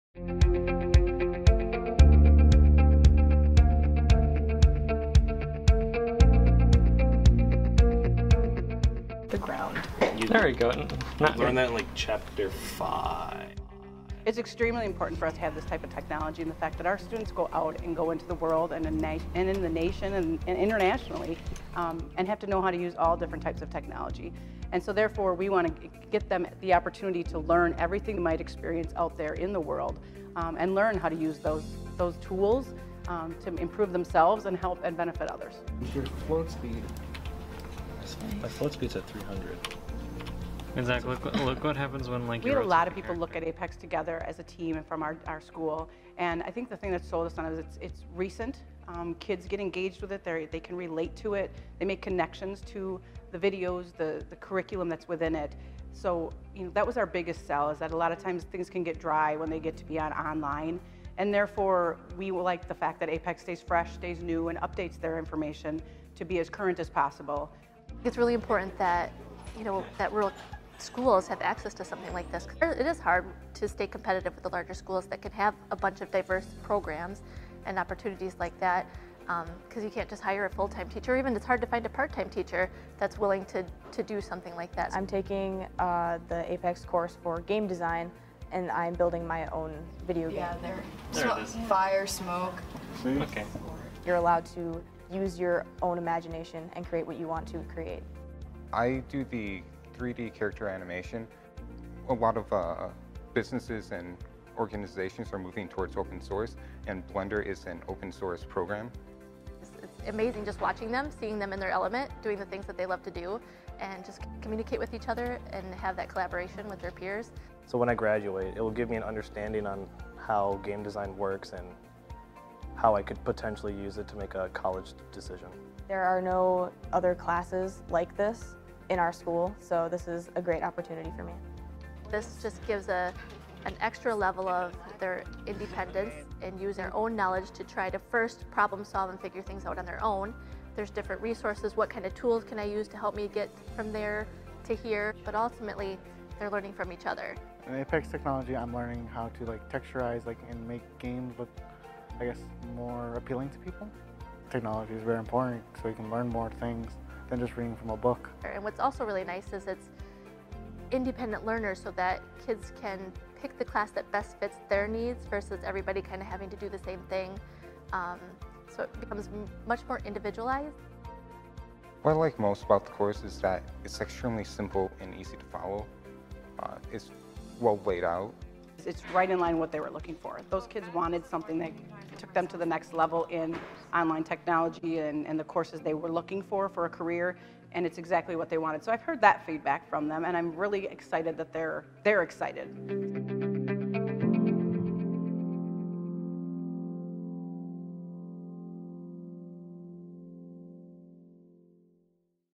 Technology Courses Prepare Students for the Future at Wautoma HS
Submission to the 2019 Learning Impact Awards.